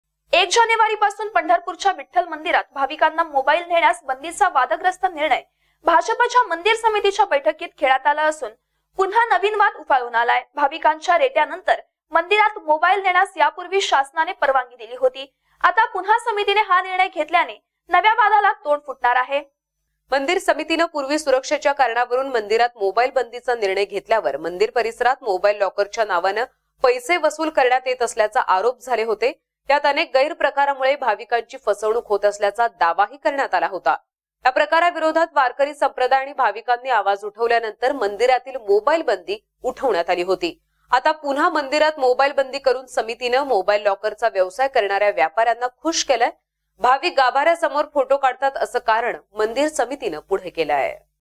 पंढरपूर विठ्ठल मंदिरात १ जानेवारीपासून मोबाईल बंदी.
भाजपच्या मंदिर समितीचा निर्णय,
नव्या वादाला तोंड फुटण्याची शक्यता.